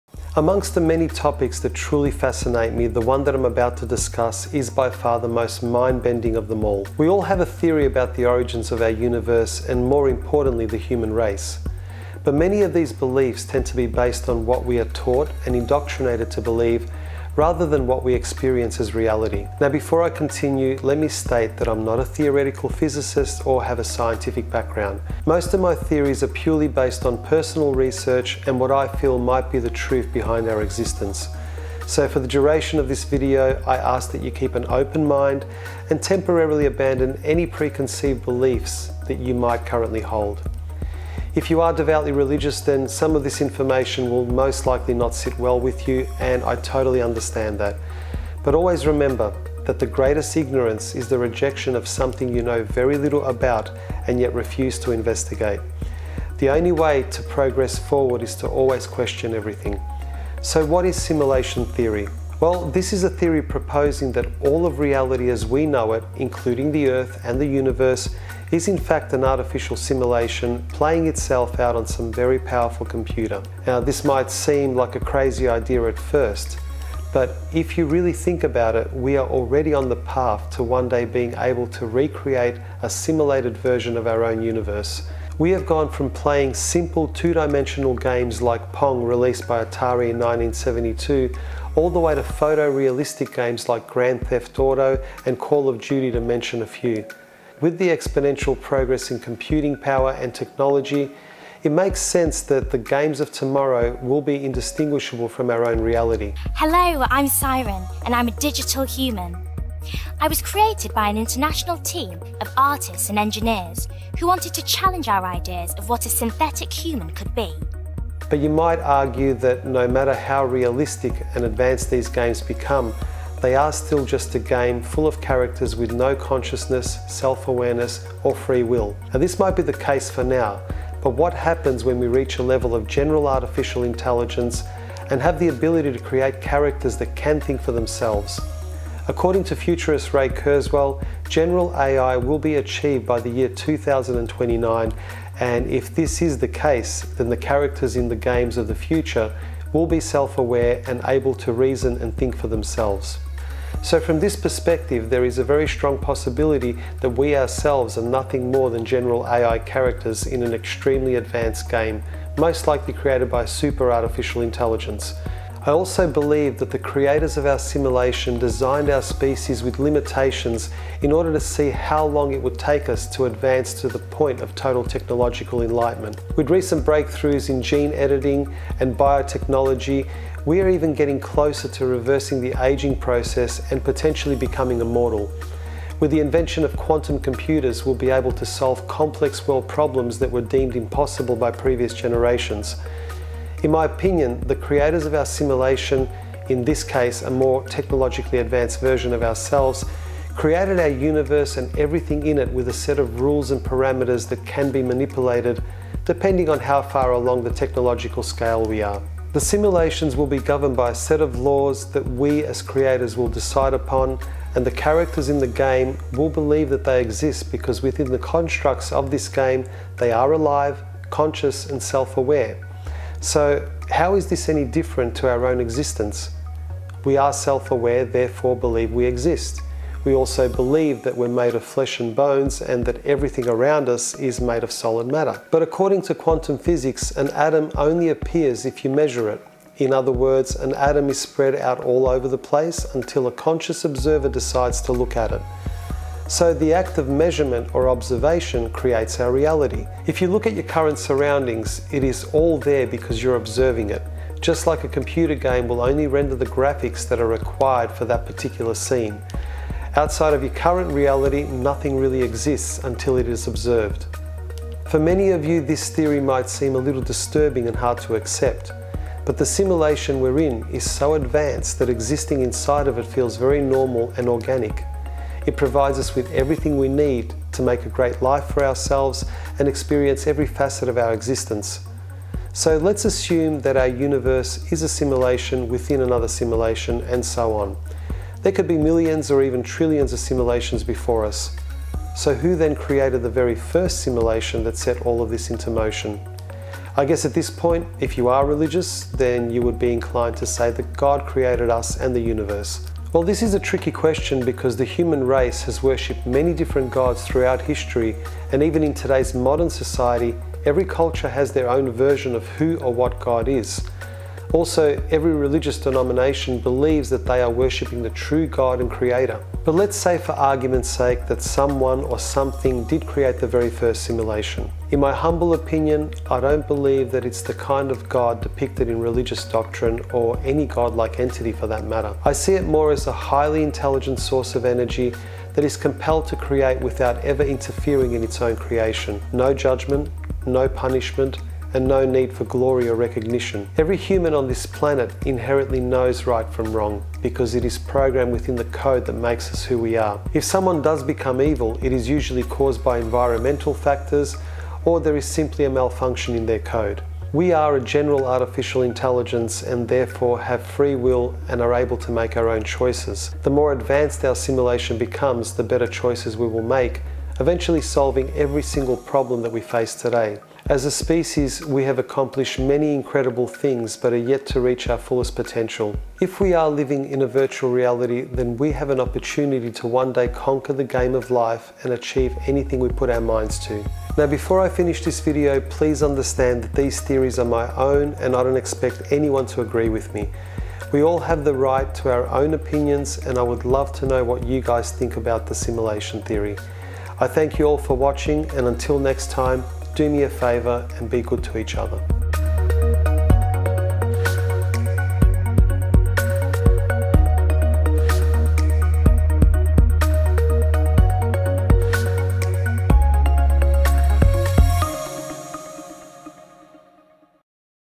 YOU ARE LIVING IN A SIMULATION!
What if the world you live in is nothing more than computer code on a very powerful computer? According to recent scientific discoveries, this very idea is a real possibility.